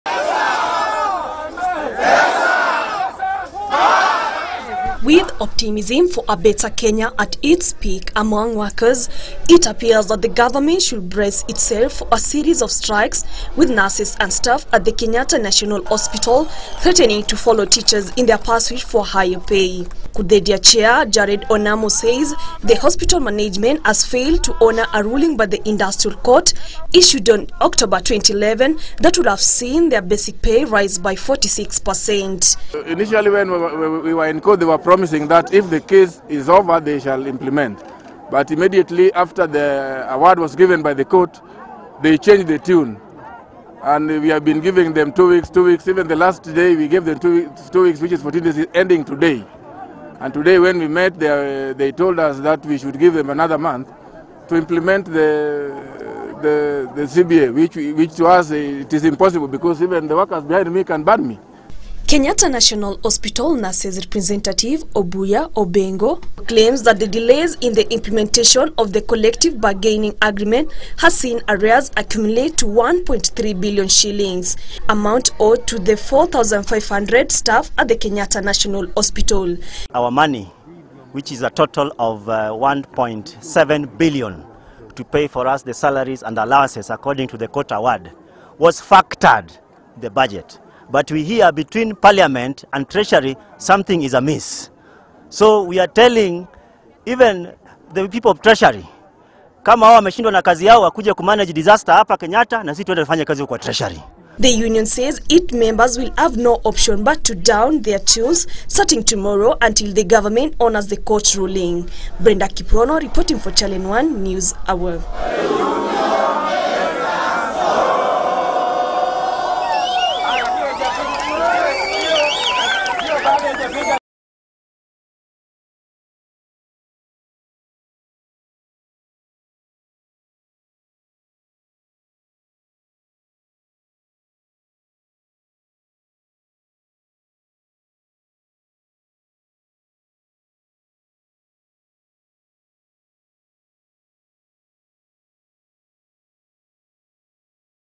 KNH nurses threaten to go on strike
As the government grapple with a looming teachers' strike, another industrial action seems to be in the offing with nurses and staff at the Kenyatta national hospital threatening to down their tools. Kenya Union of Domestic, Hotel, Educational Institutions, Hospitals, and Allied Workers is accusing the hospital management of failing to implement  a collective bargaining agreement it had entered with the union.